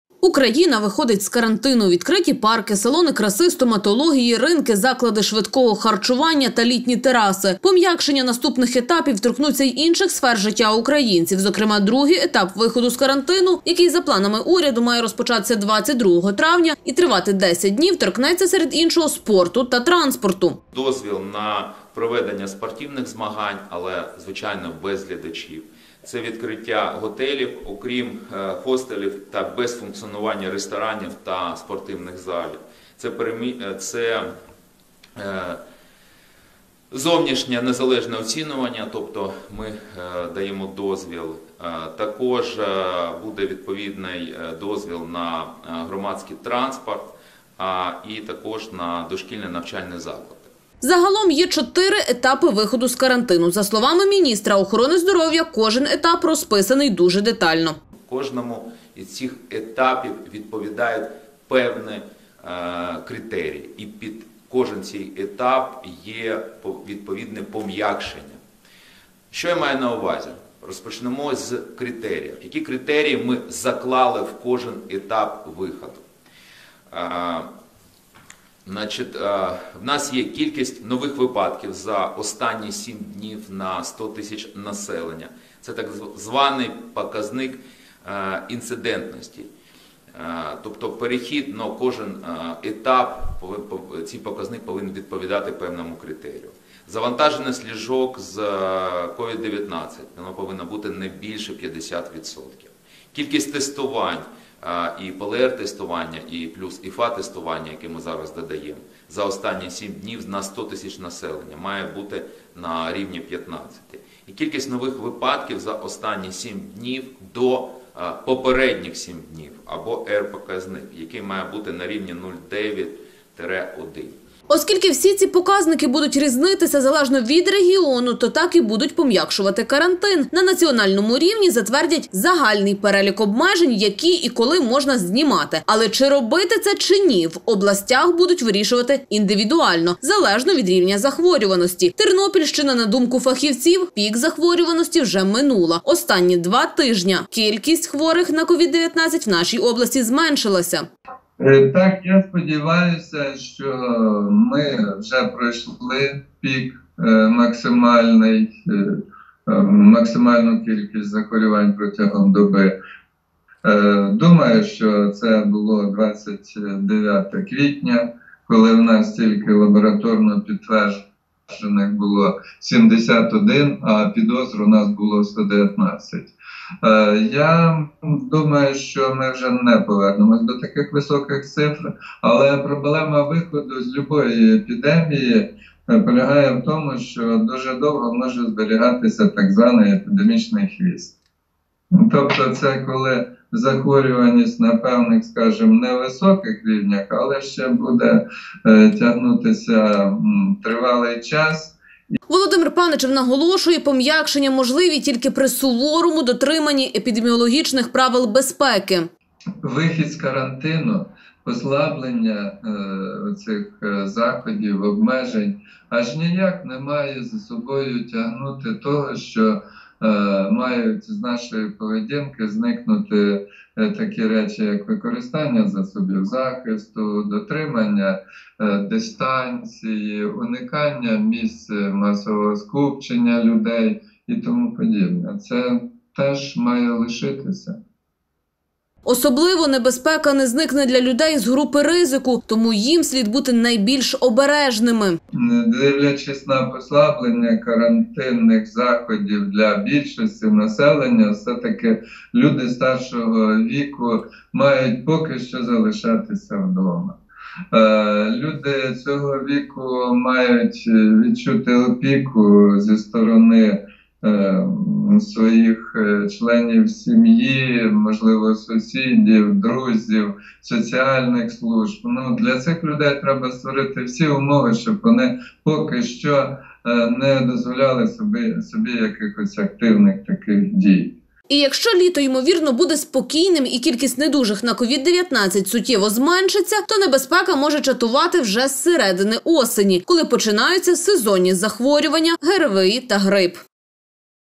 Користуватися громадським транспортом тернополяни зможуть з 22-го травня
Україна виходить з карантину.  Відкриті парки, салони краси, стоматології, ринки, заклади швидкого харчування та літні тераси. Пом’якшення наступних етапів торкнуться й інших сфер життя українців. Зокрема, другий етап виходу з карантину, який за планами уряду має розпочатися 22-го травня і тривати 10 днів, торкнеться, серед іншого, спорту та транспорту.